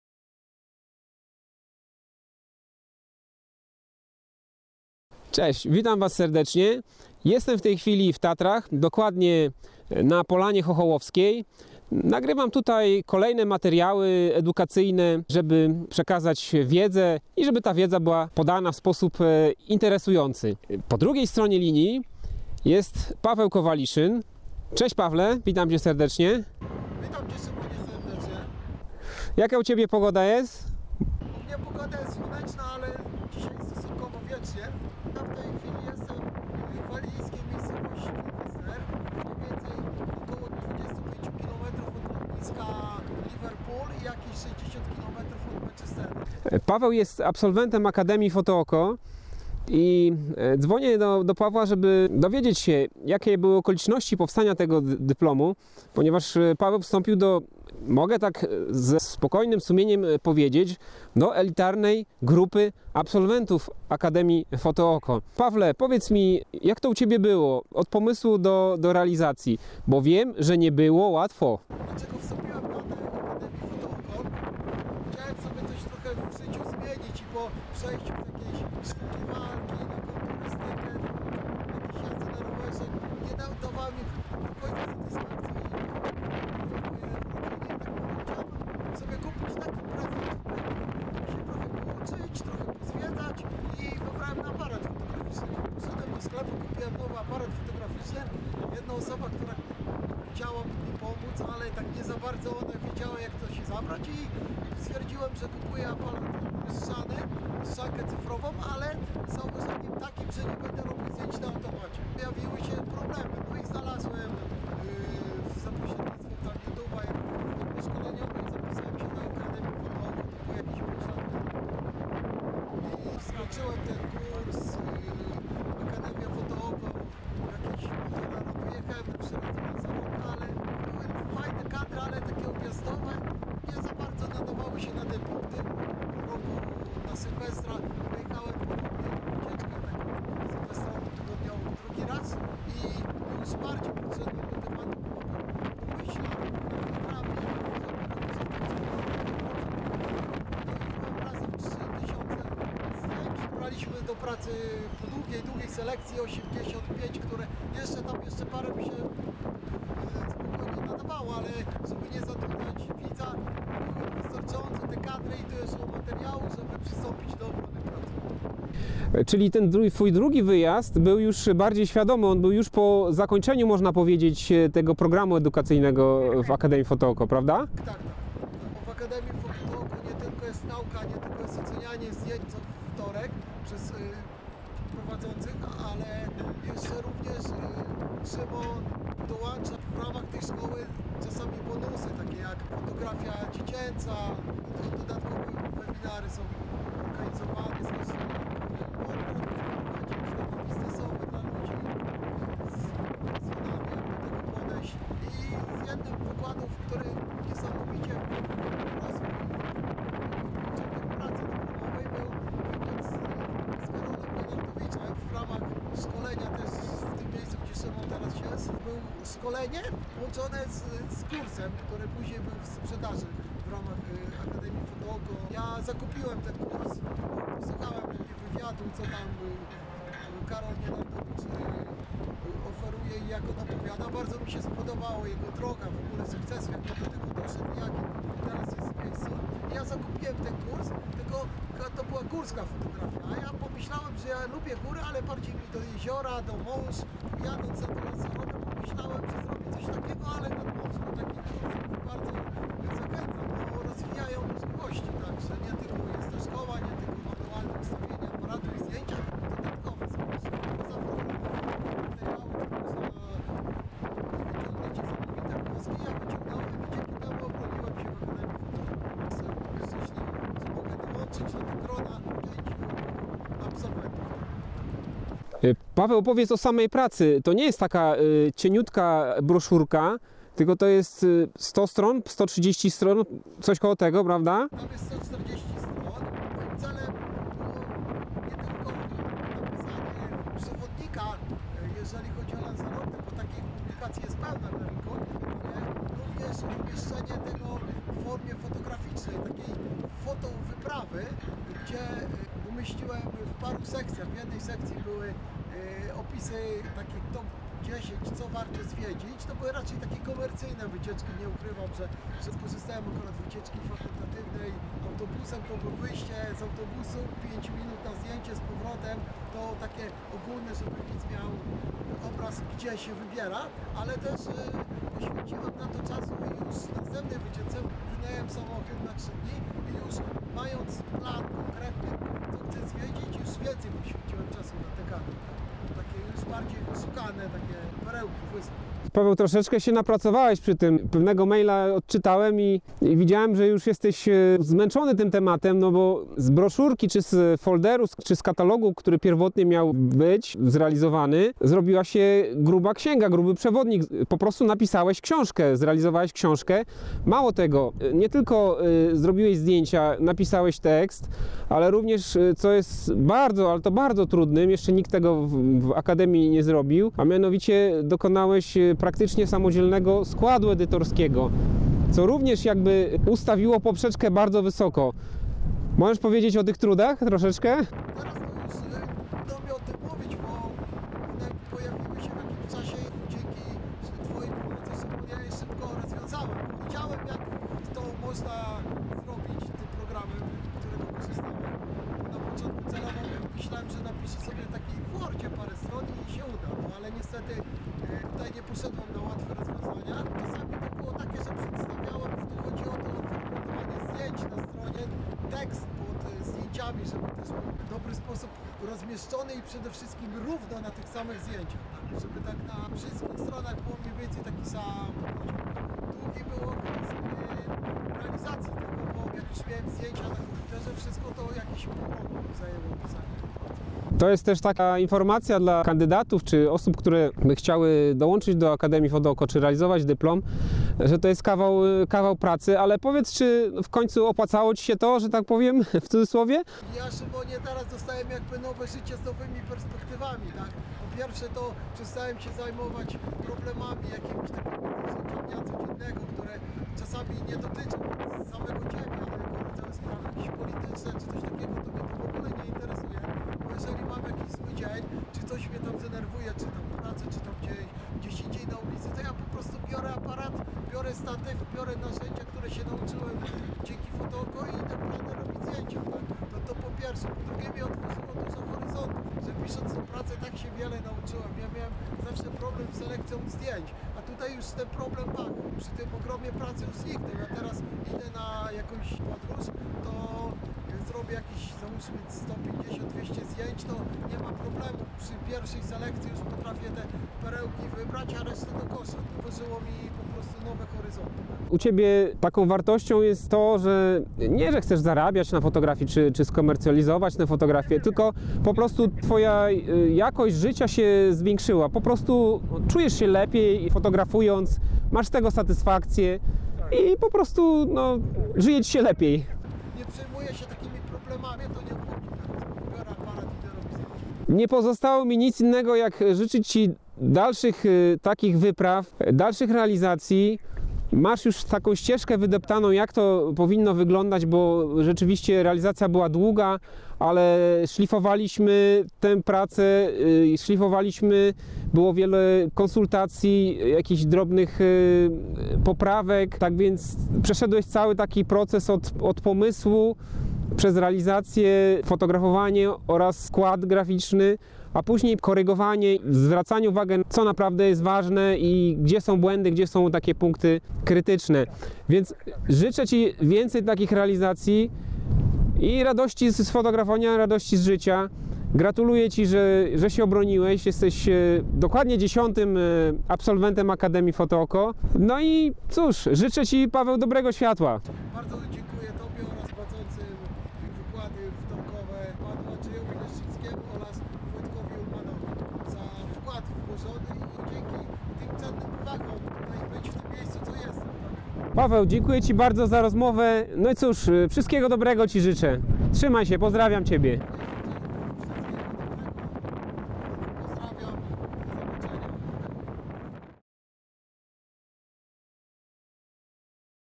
Foto-wyprawa Lanzarote . Wywiad z absolwentem Akademii Foto Oko Pawłem Kowaliszynem
Wywiad z absolwentem Akademii Foto Oko Pawłem Kowaliszynem. Otym, jak wykonał 140. stronicową e-publikację o jednej z wysp kanaryjskich - Lanzarote.